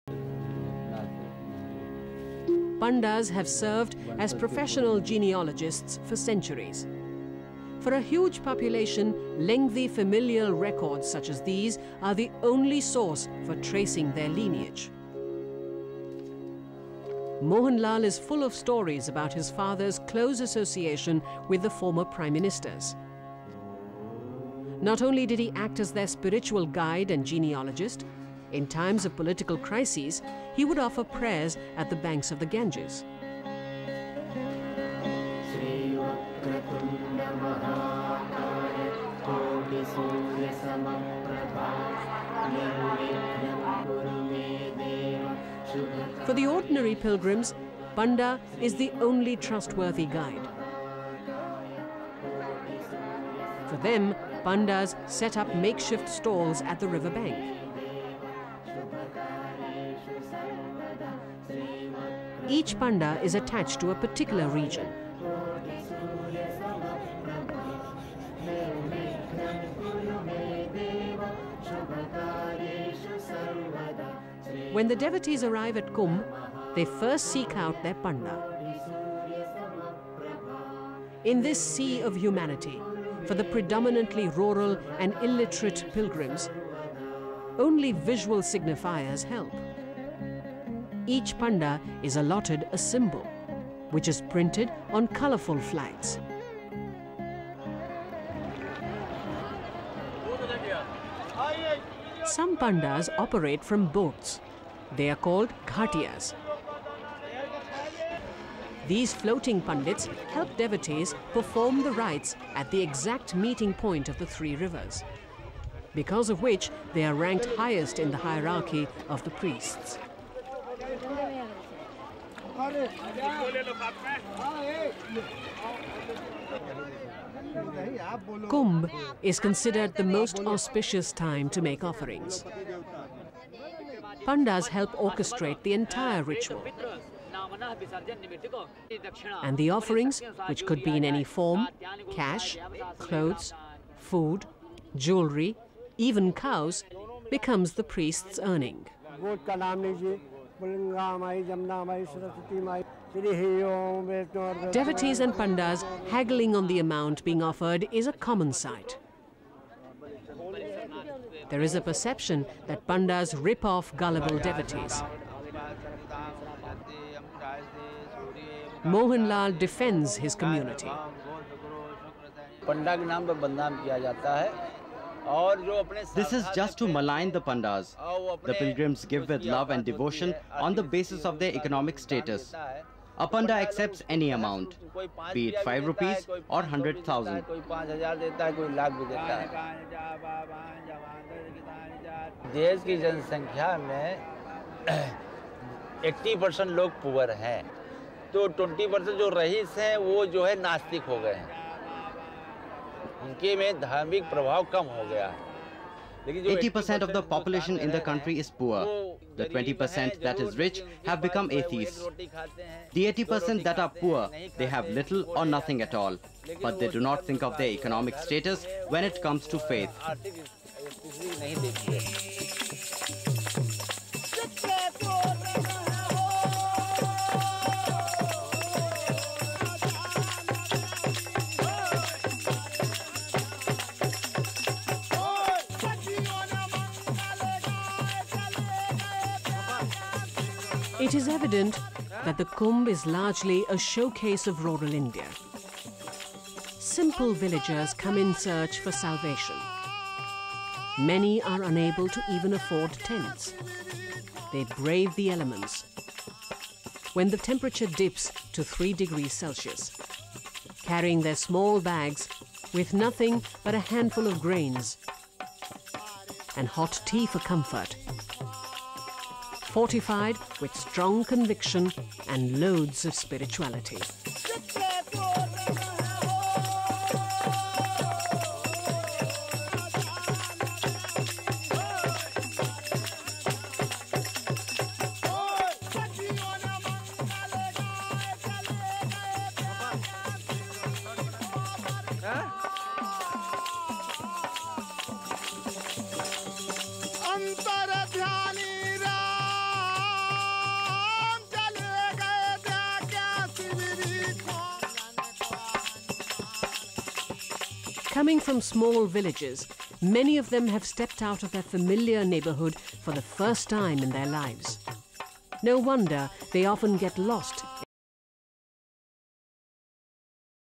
Pandas or professional genealogist during Kumbh Mela, Allahabad
Pandas have served as a professional genealogist for centuries. For a huge population, lengthy familial records such as these are the only source of tracing their lineage.

Mohan Lal is full of stories about his father's close association with the former Prime Minister not only did he acted as spiritual guide and genealogist, in times of political crisis he would offer prayers at the banks of the Ganga. For the ordinary pilgrims, Panda is the only trustworthy guy. For them, Pandas set up makeshift stalls at the river bank. Each Panda is attached to a particular region. When the devotees arrive at Kumbh they seek out their Panda. In this sea of humanity for the predominantly rural and illiterate pilgrims, only visuals signify help each panda is alloted a symbol which is printed on colorful flags.